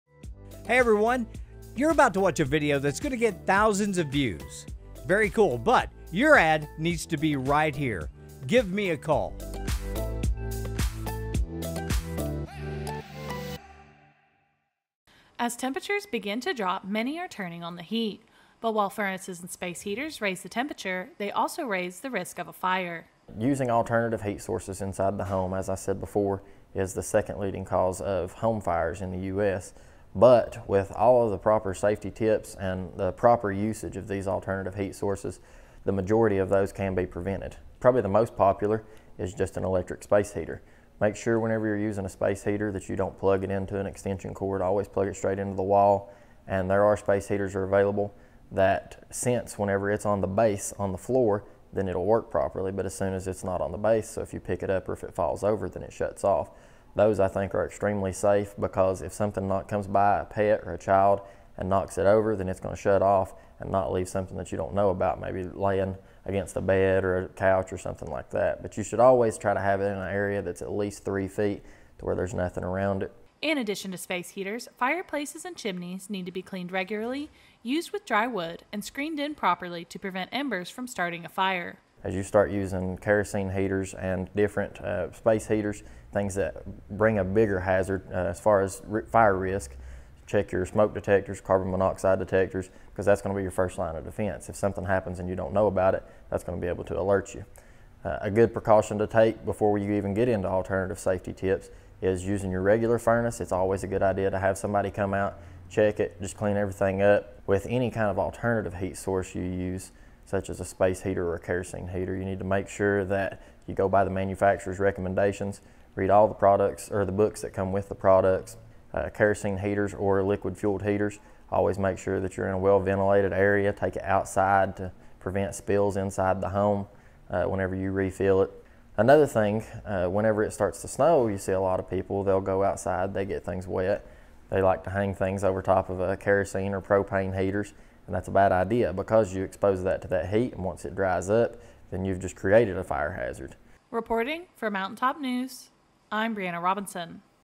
Keeping Warm and Safe While Temperatures Drop
As temperatures drop, many will be tempted to turn up the heat.  While it can be comforting, it can also create a higher risk for fire.